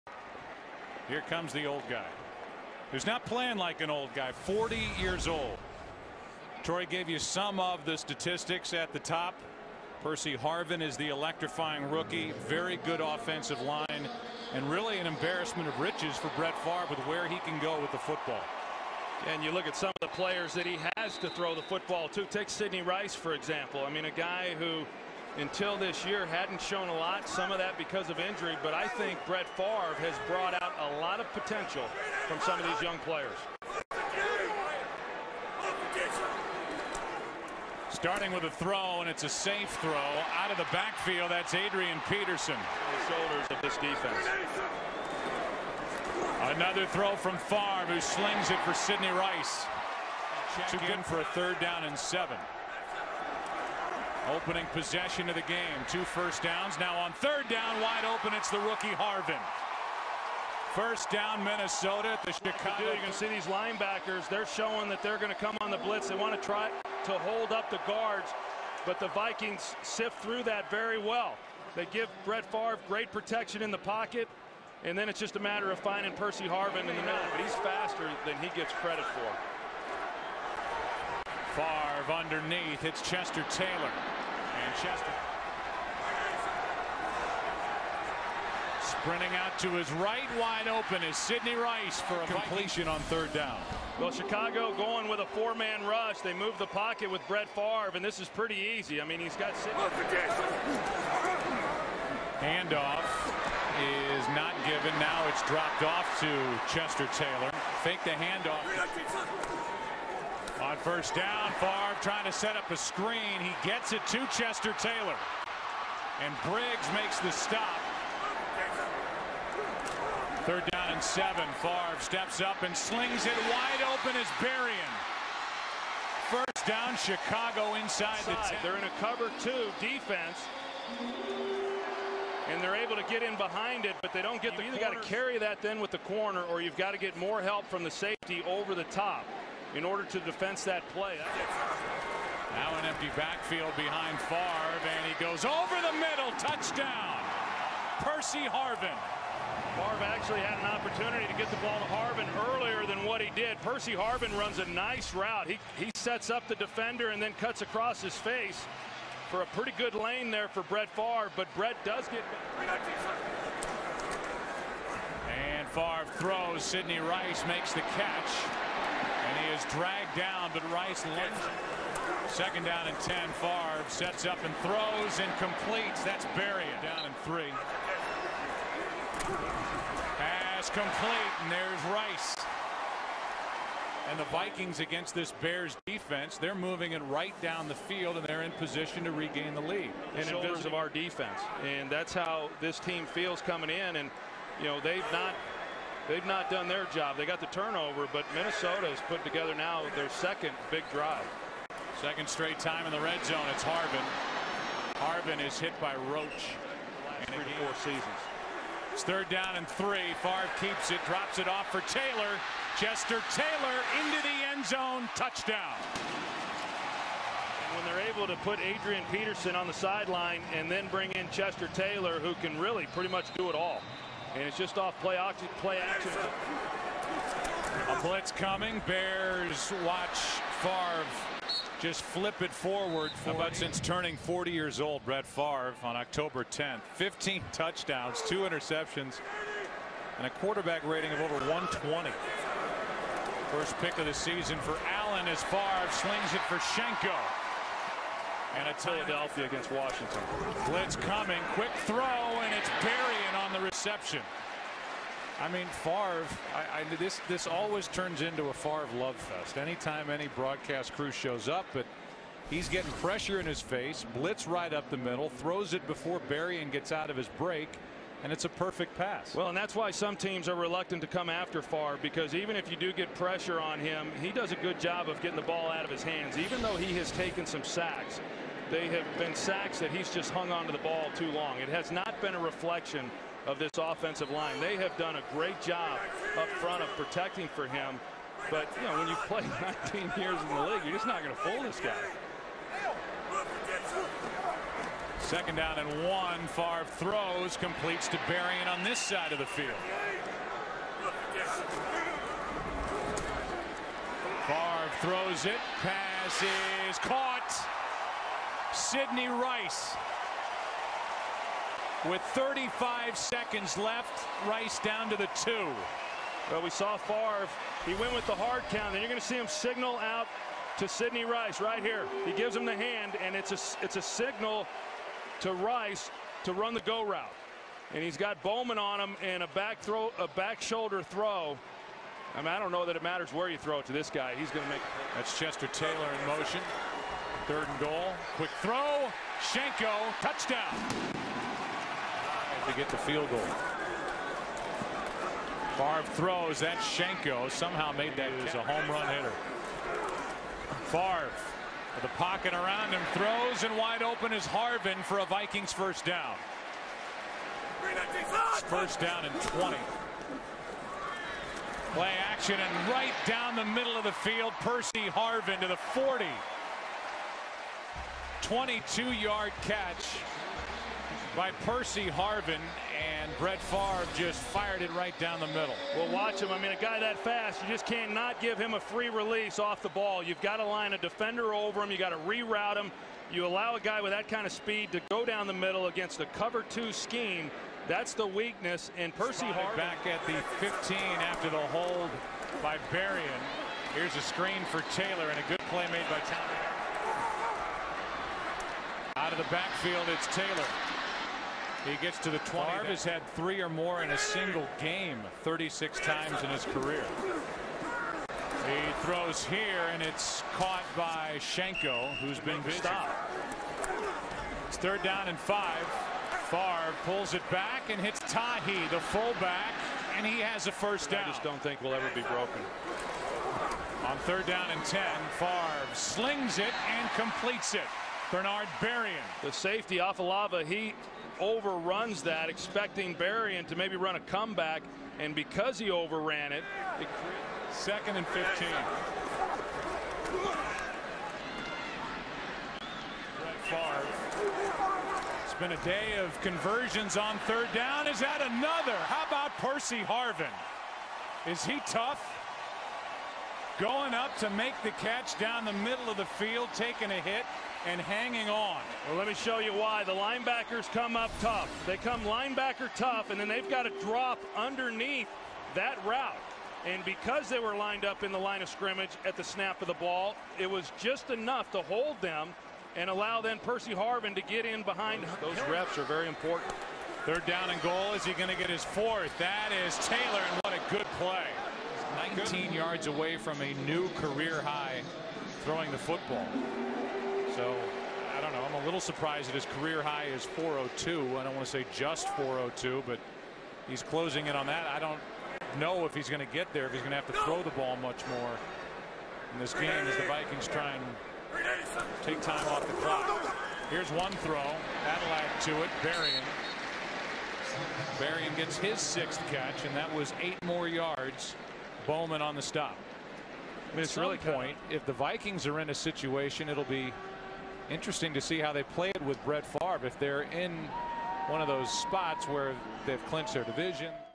Brett Favre See's Himself In Patrick Mahomes | Throwback Highlights 11.29.2009
Brett Favre had his best year as a pro towards the end of his career. Recently he made comments about Patrick Mahomes complimenting him on his play and I think that is largely due to the similarities in their games. Here I show you Brett Favre's 2nd most passing yards in a game as I'll let you be the judge of the two. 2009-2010 Season

STATS: 32/48 392YDS 3TD